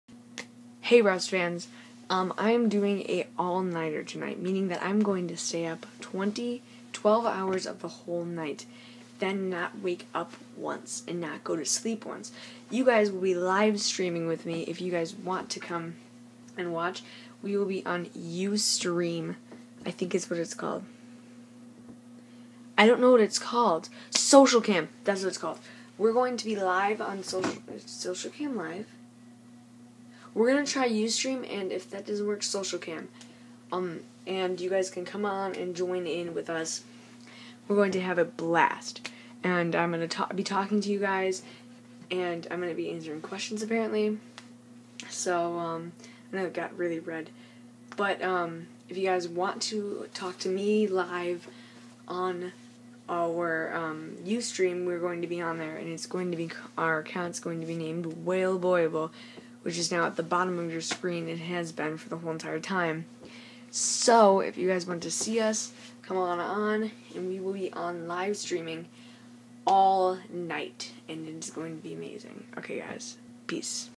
Matt st. Marie is going live
Tonight starting at 11:00 PM Central time,
Matt will be Ustreaming live!
Our account name will be Wailboyable so join us on the fun!